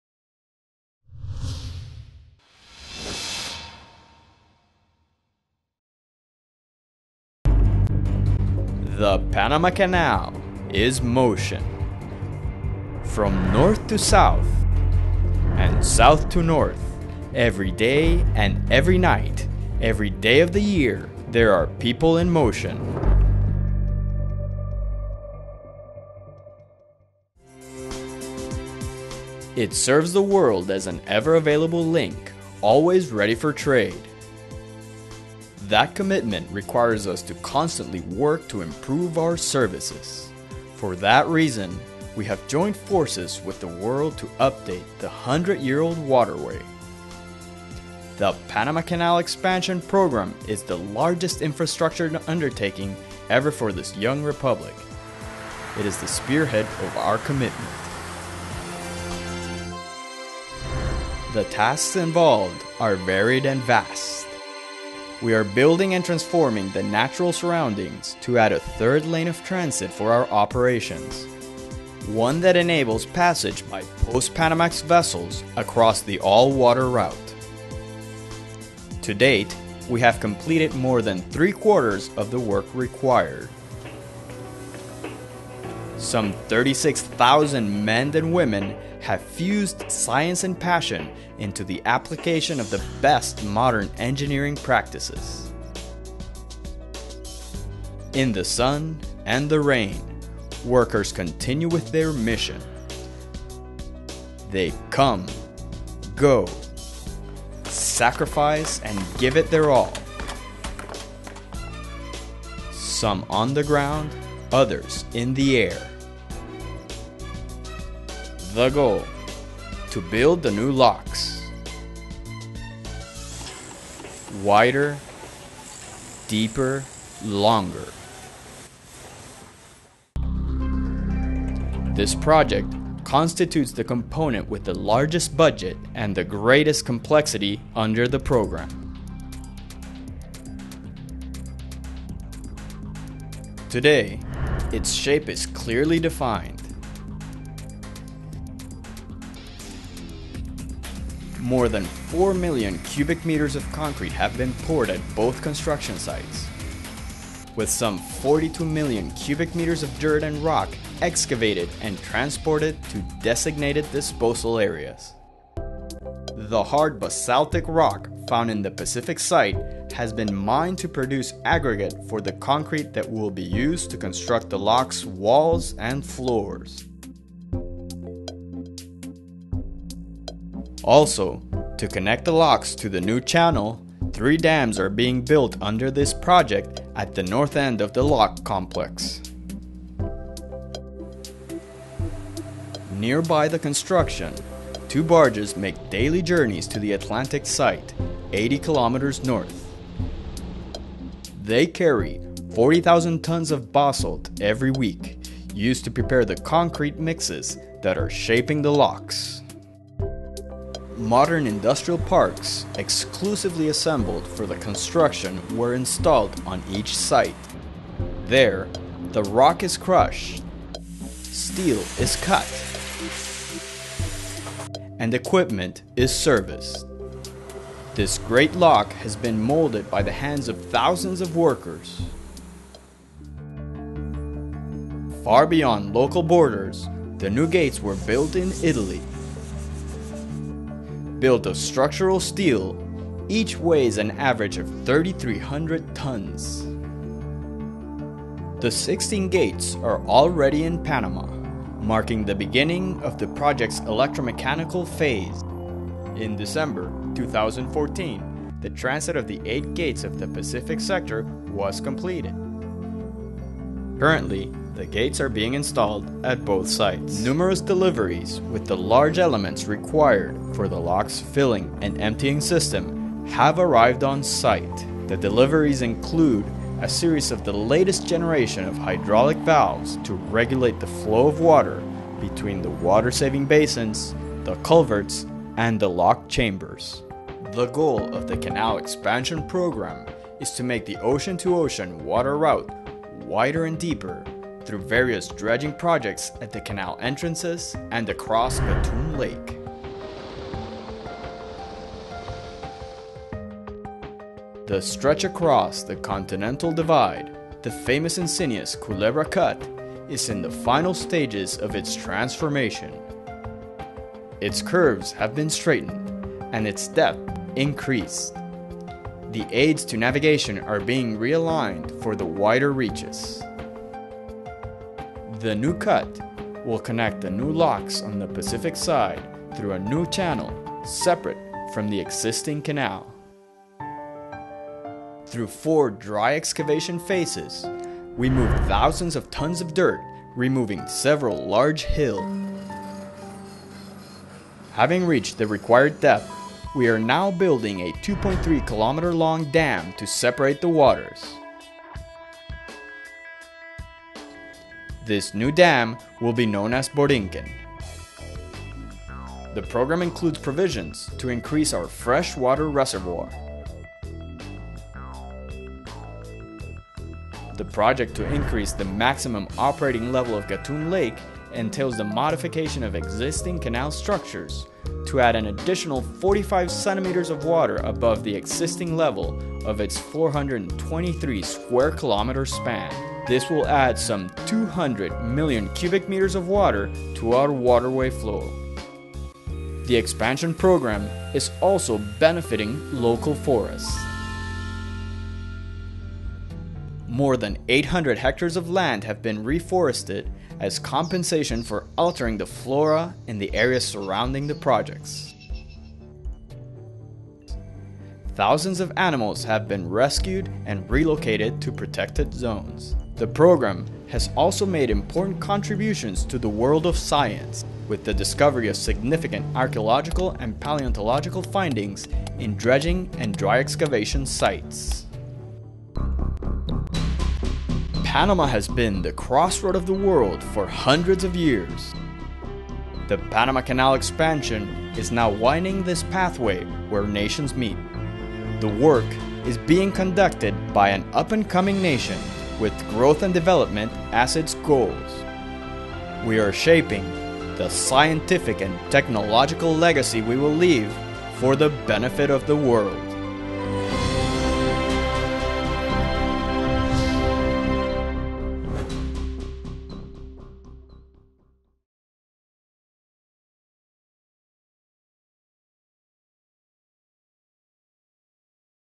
Panama Canal Expansion Program Update - March 2015
The largest infrastructure project since the waterway’s original construction, the Panama Canal Expansion Program will create a new lane of traffic along the Canal through the construction of a new set of locks, doubling the waterway’s capacity. The new locks will have three chambers, water-saving basins, lateral filling and emptying system & rolling gates.

Learn more about this project at expansion.pancanal.com